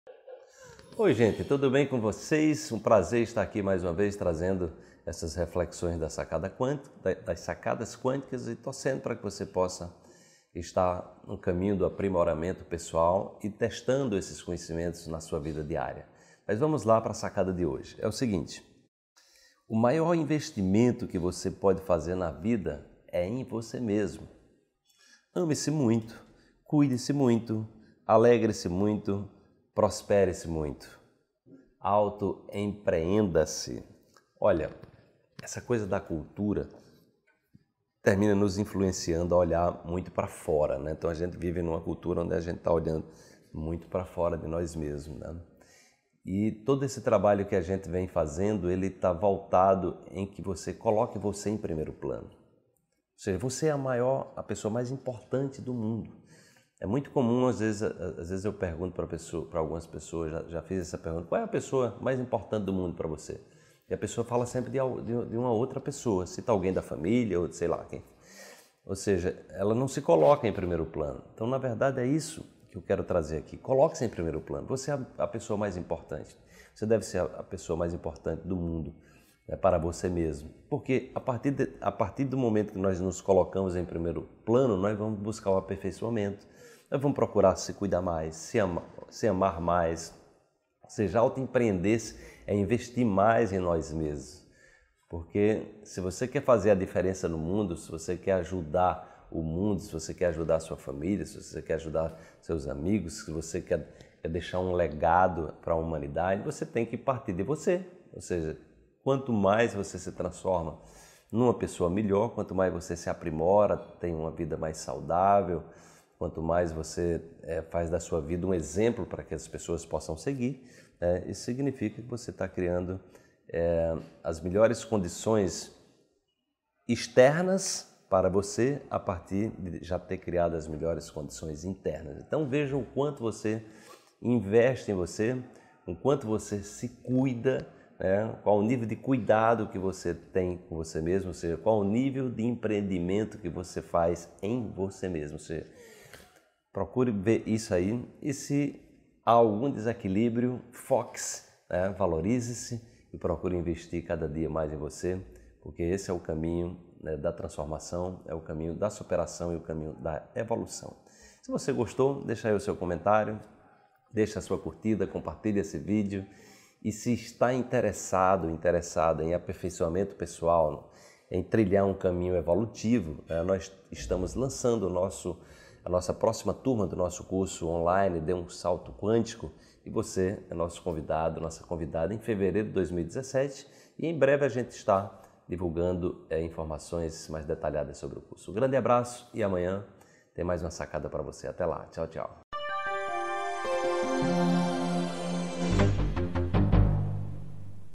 Coaching Quântico 233: Invista em Você Mesmo, Autoempreenda-se! | Wallace Liimaa
✭✭✭✭✭ Nesse vídeo abordo a importância de colocar-se em primeiro lugar no plano da vida, amando-se, empreendendo-se e empoderando-se. Assista já